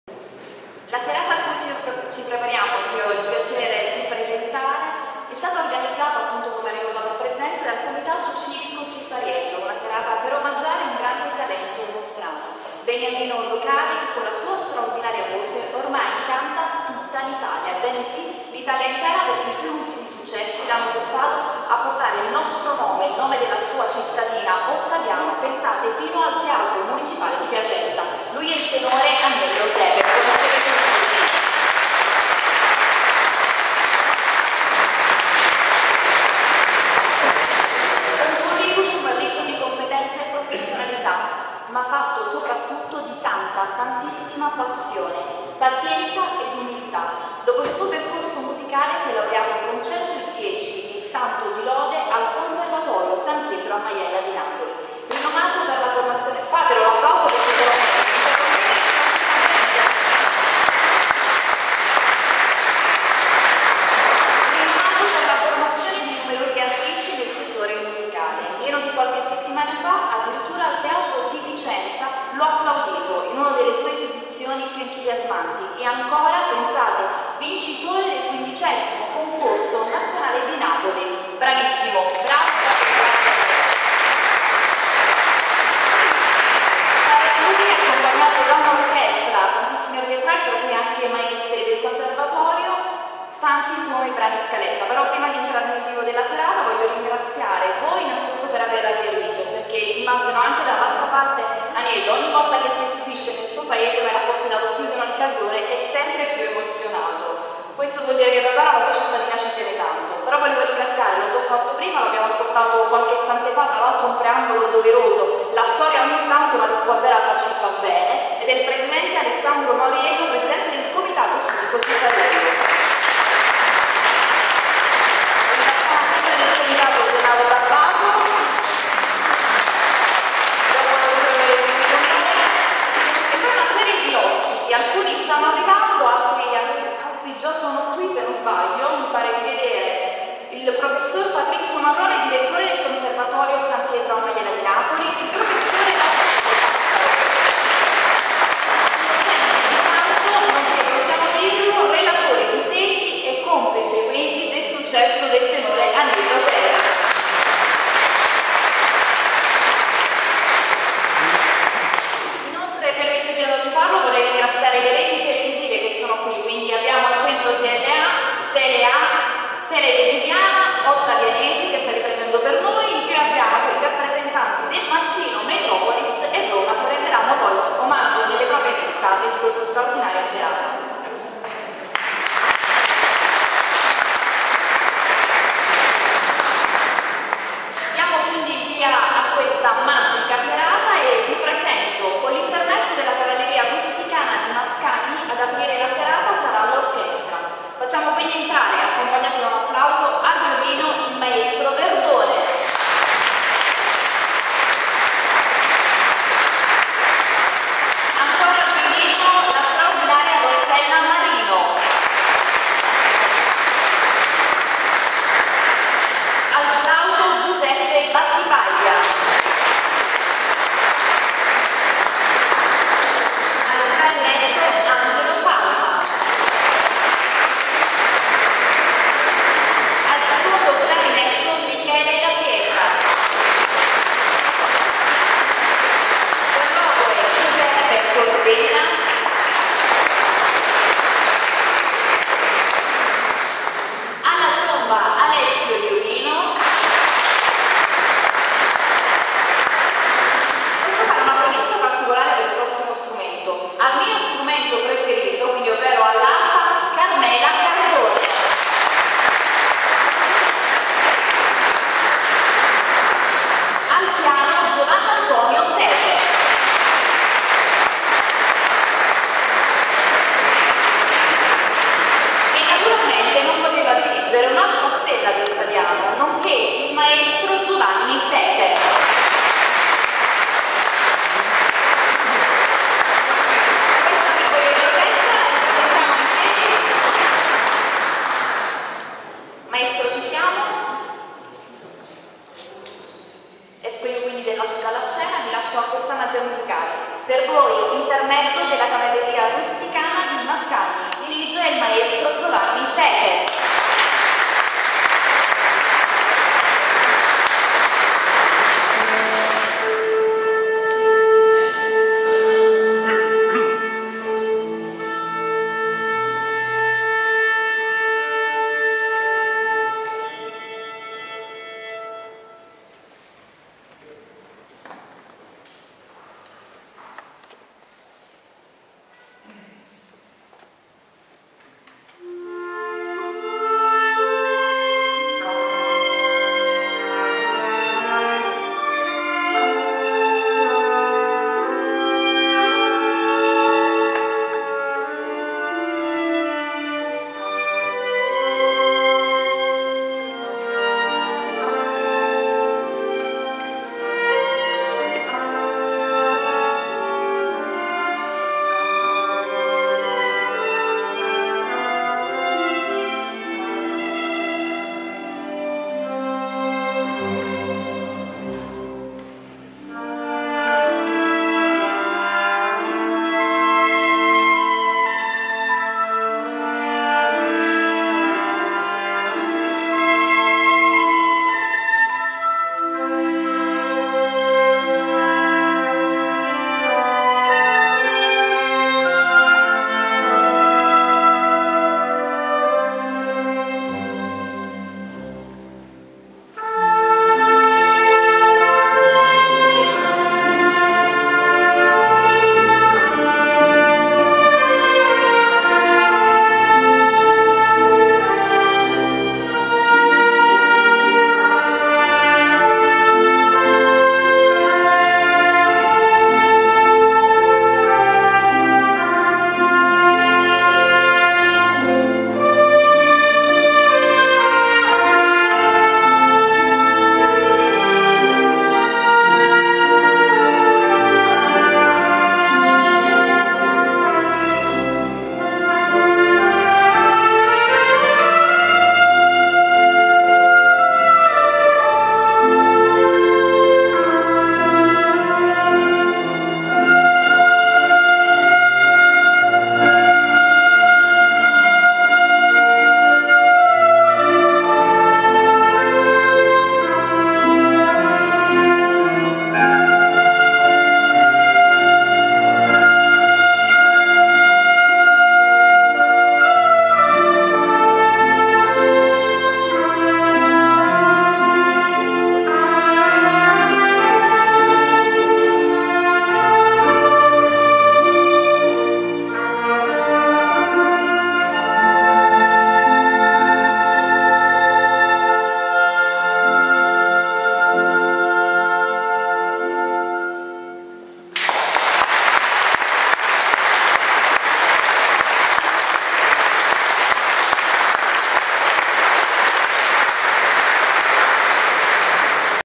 TenoreAnielloSepe 03 di 10
Sabato 19 giugno 2010 nella Chiesa del SS. Rosario in Ottaviano. Reduce dal successo ottenuto al Teatro Municipale di Piacenza e dopo la prestigiosa vittoria al 15° Concorso Nazionale di Canto Lirico, il Comitato Civico "A.Cifariello"-ONLUS, ha organizzato una serata di canto e musica in onore del tenore Aniello Sepe. Con la partecipazione del Maestro Raffaele Passaro - Docente di canto al Conservatorio "San Pietro a Maiella" di Napoli. presenta la giornalista Giovanna Salvati.